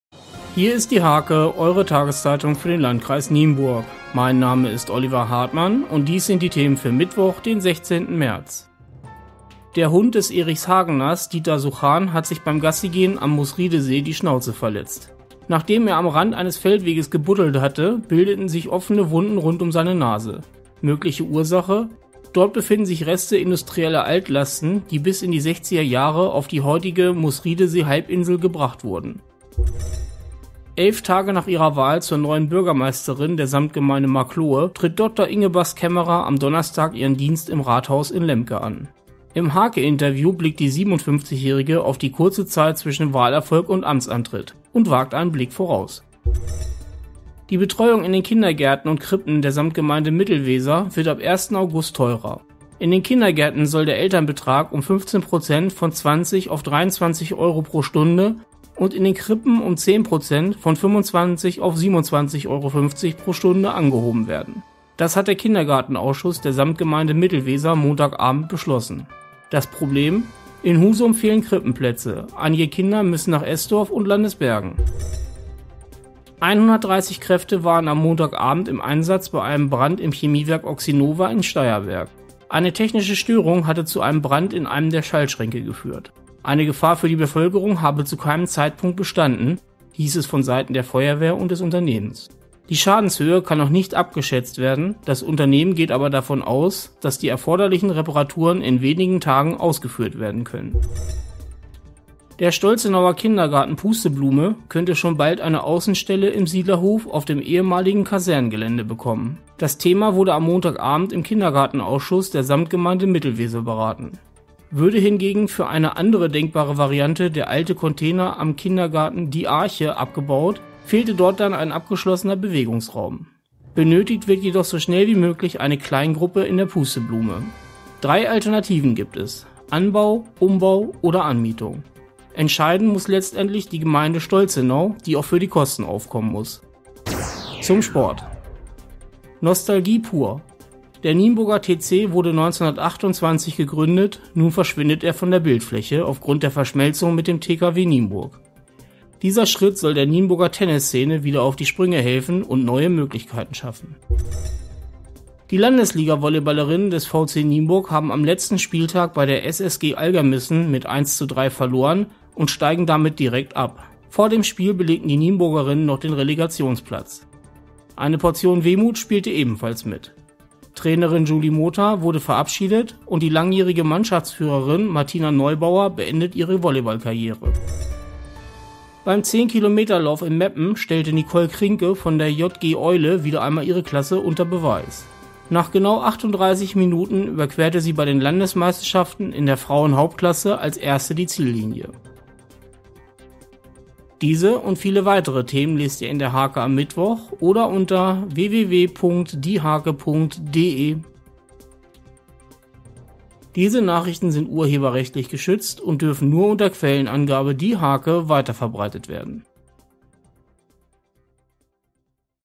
DIE HARKE vom 16.03.2016
21 Uhr, Zeit für eine neue Ausgabe der HARKE Video-News. Die Themen für Mittwoch, den 16. März, lauten:

- Verletzte Schnauze durch Altlasten?
- Dr. Inge Bast-Kemmerer startet
- Kindergartengebühren steigen um bis zu 15%
- Feuer im Chemiewerk Oxxynova
- Außenstelle für „Pusteblume“
- Abschied vom Nienburger TC
- Abstieg für VC Nienburg II
- Einzelgold für Nicole Krinke